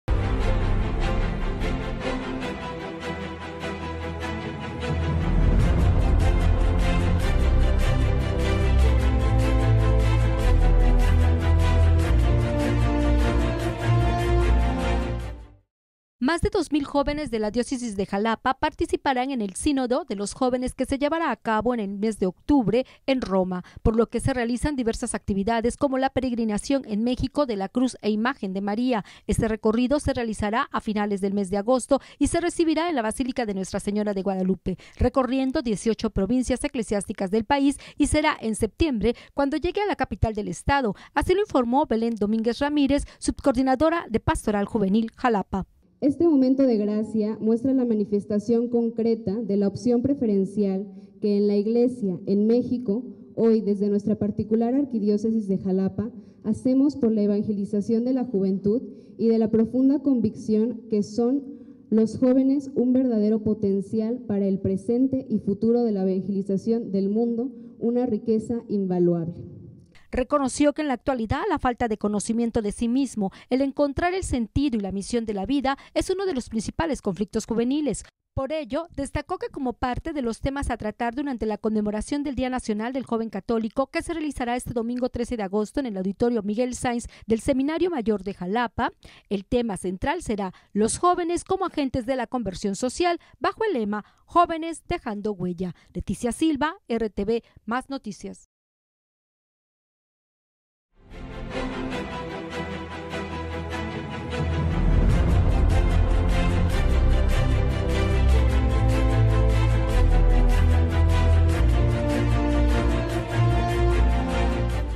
Jóvenes de Xalapa peregrinarán a la Basílica de Guadalupe previo a visita a Roma
Más de dos mil jóvenes de la Arquidiócesis de Xalapa participarán en del Sínodo de Jóvenes que se llevará a cabo en octubre próximo en Roma, por lo que se realizan diversas actividades como: la peregrinación en México de la cruz e imagen de María.
Este recorrido se realizará a finales de agosto y se recibirá en la Basílica de Nuestra Señora de Guadalupe, recorriendo  18 provincias eclesiásticas del país y será en septiembre cuando llegue a la capital del estado informo Belem Domínguez Ramírez, subcoordinadora de pastoral juvenil.